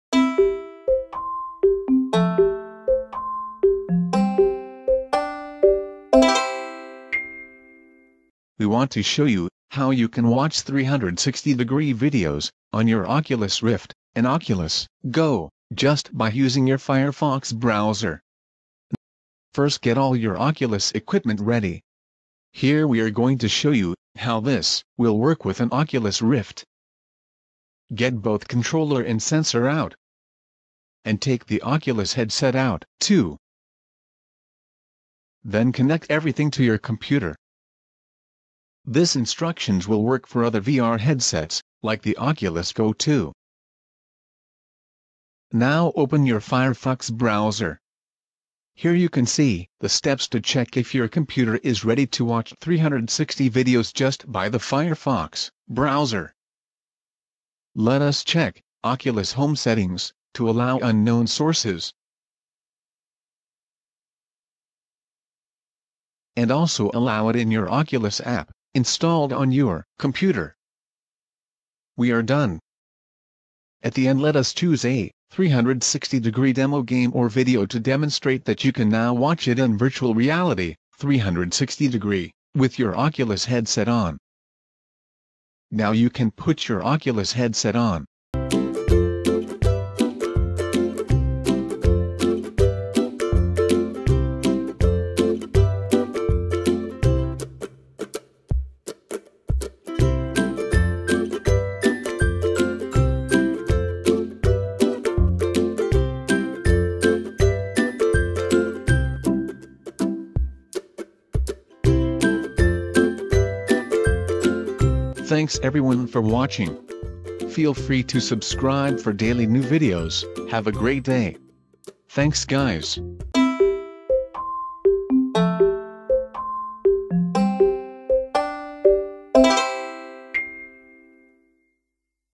How to watch 360 videos with Firefox WebVR - Oculus Rift & Oculus Go
Quick tutorial video on how to watch 360 degree videos just by your Firefox browser by WebVR.

Oculus Rift Virtual Reality headset

Filmed with Samsung Galaxy S10 smartphone

All products we used on our channel: